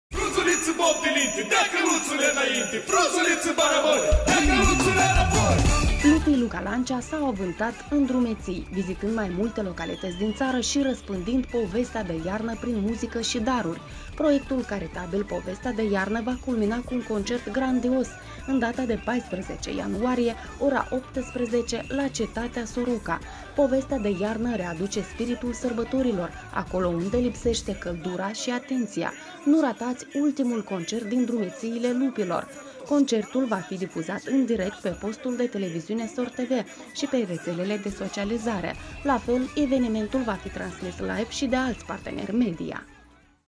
„Povestea de iarnă” la cetatea Soroca
„Povestea de iarnă”  va culmina cu un concert grandios în data de 14 ianuarie, ora 18:00, cetatea Soroca.